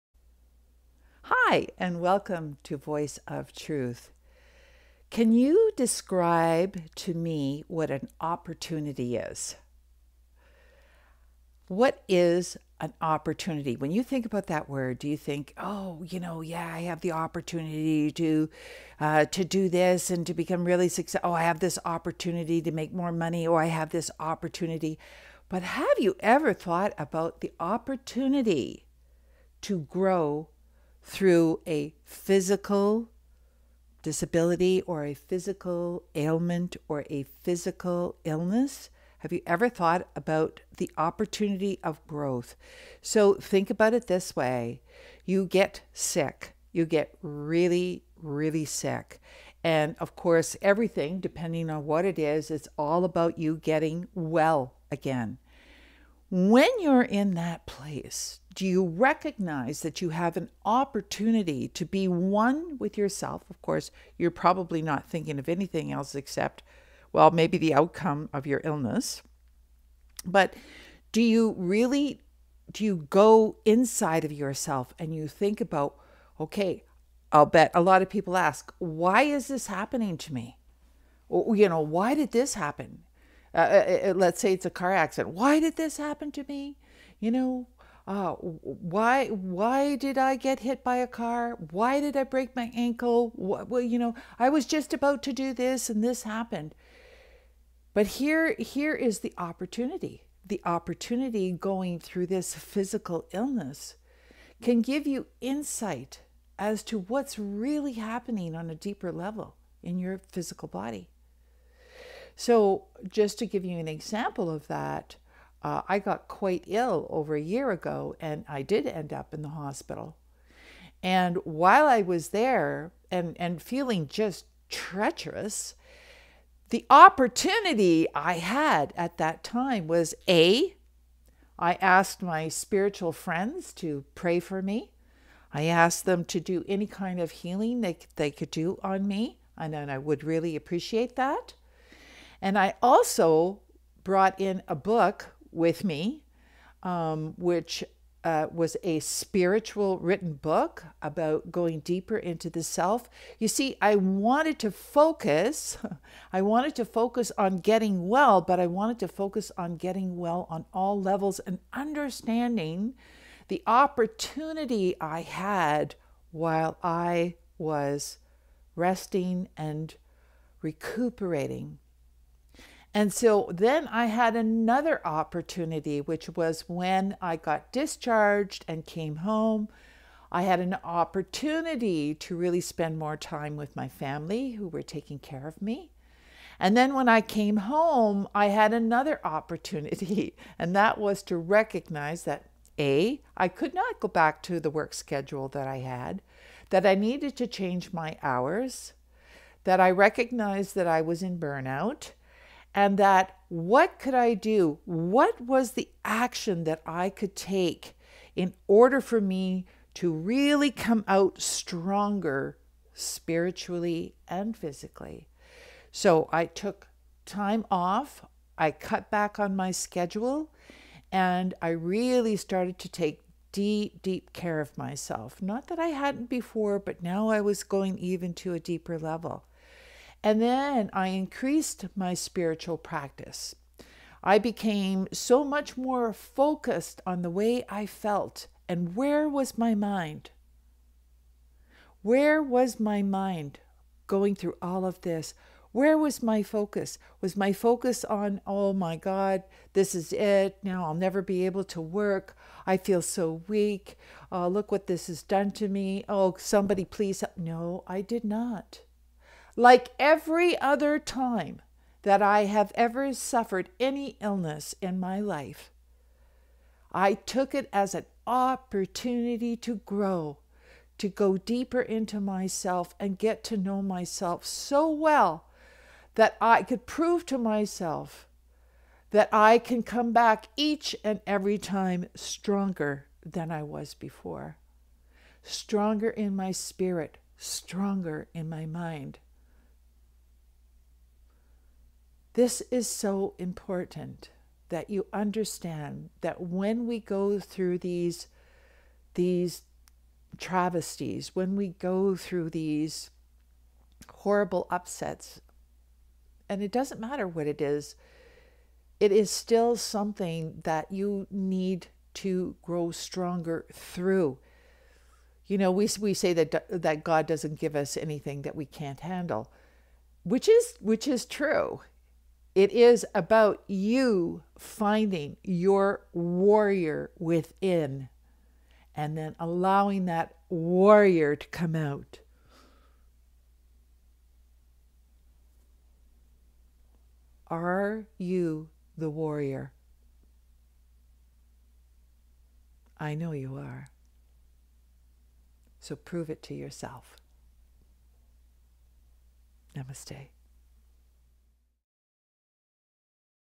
Voice of Truth: Ep. 49 - Opportunity
When I say to you ‘Opportunity’, what comes up for you?

Have you ever thought about the opportunities that come through for you during a difficult time or an illness?

A lot of people will ask, ‘Why did this happen to me?”, and that is normal.

Growth from these experiences is still there.

The next time you are in struggle, take some time to breathe and refocus, so you can understand the opportunity you have during those times.